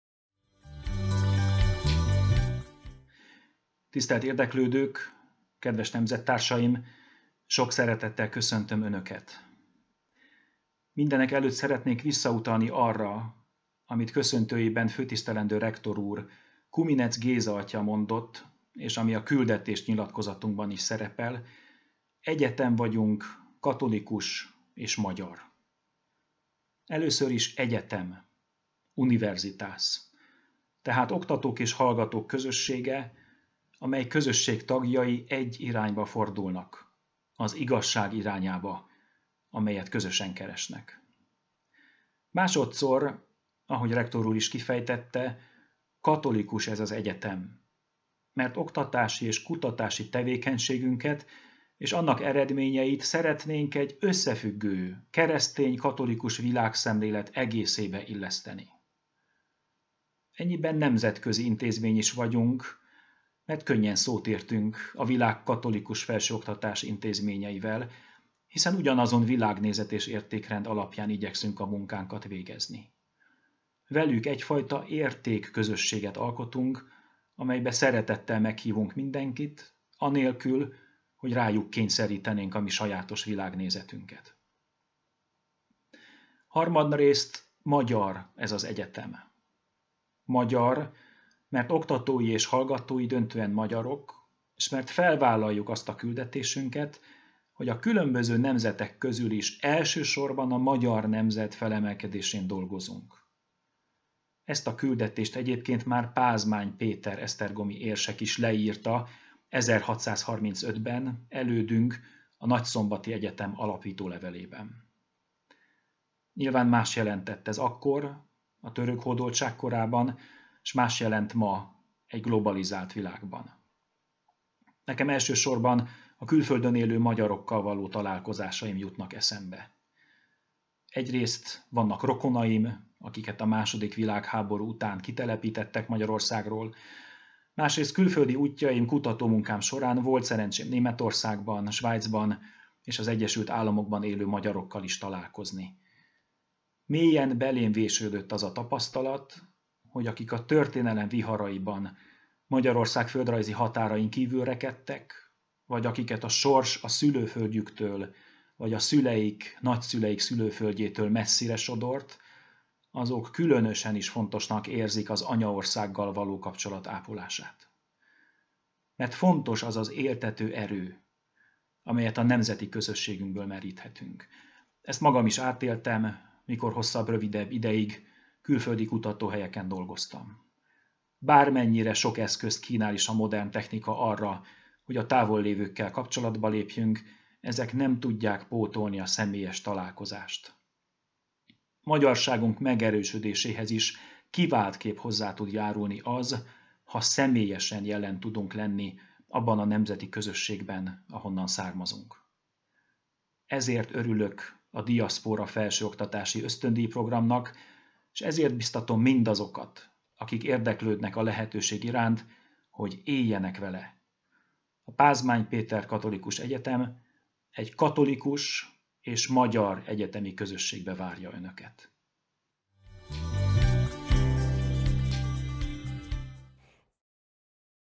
Dr. Komáromi László oktatási rektorhelyettes köszöntője
Welcome speech of László Komáromi Dr., Vice Rector for Education of the Péter Pázmány Catholic University.

The video was produced by the Strategy and Communications Department.

A videót a Stratégiai és Kommunikációs Osztály készítette.